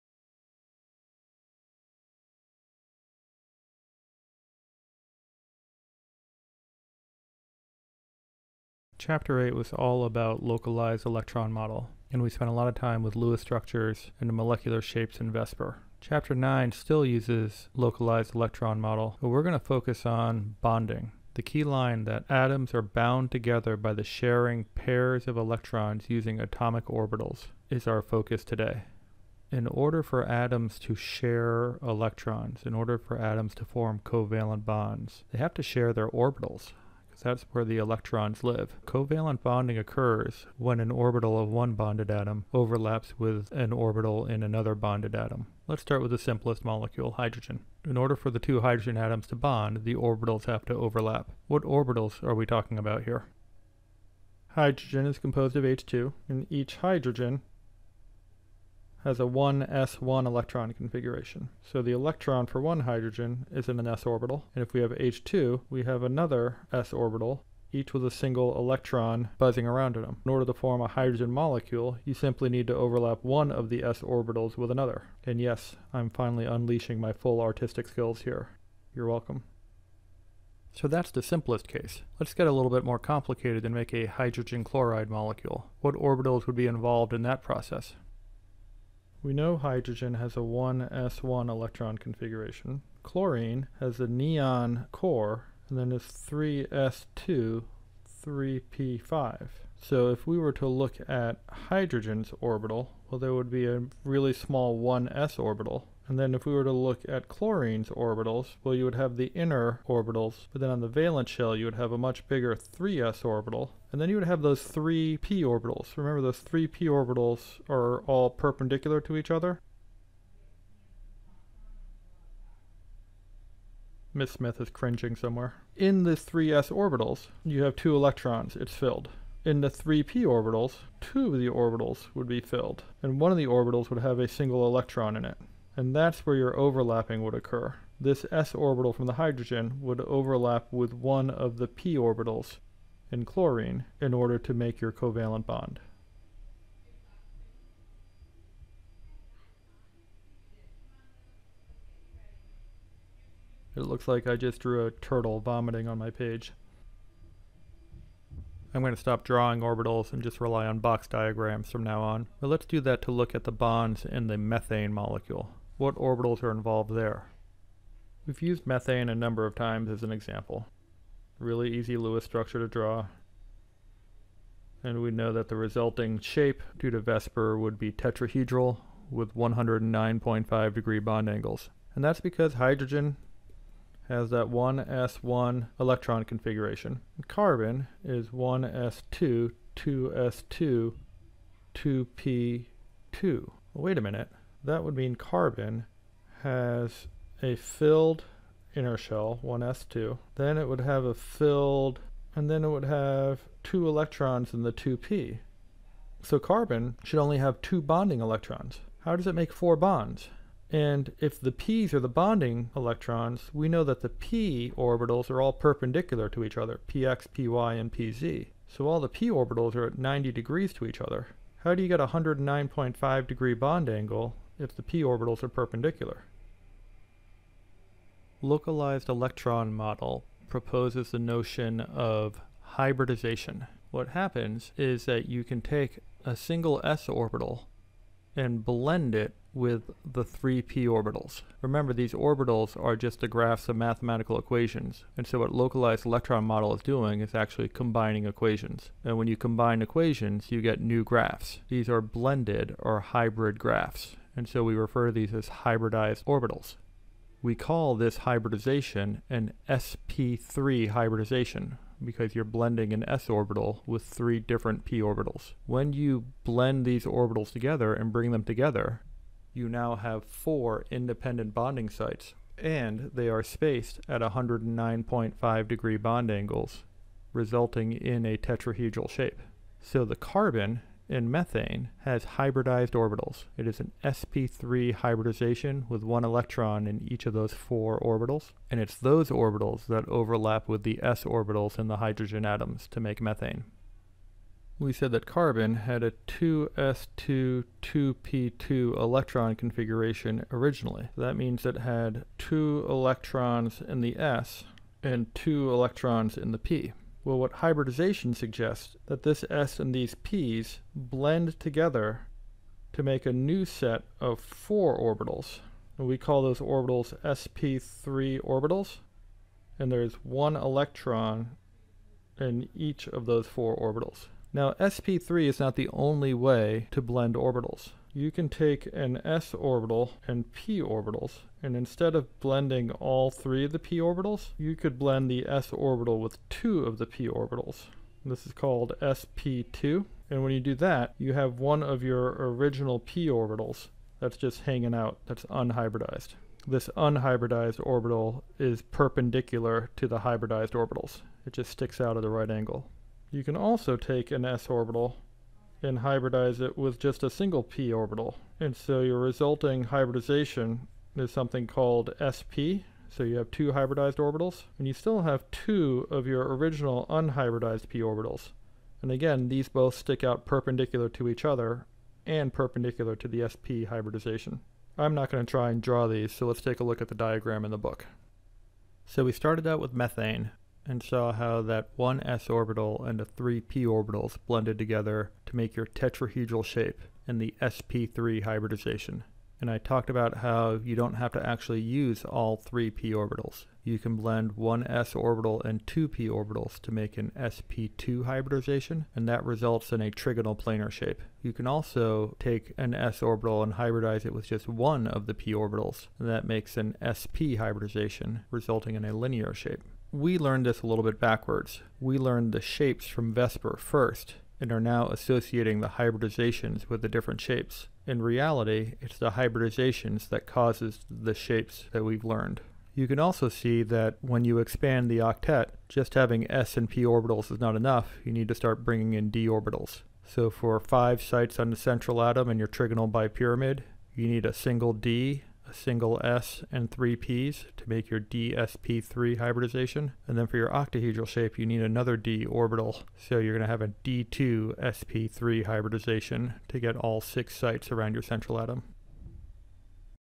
09.01 Hybridization
An introduction to the blending of orbitals to facilitate covalent bonding.

Image Credits:

Zumdahl & Zumdahl. Chemistry. 9th ed.: Cengage Learning, 2014.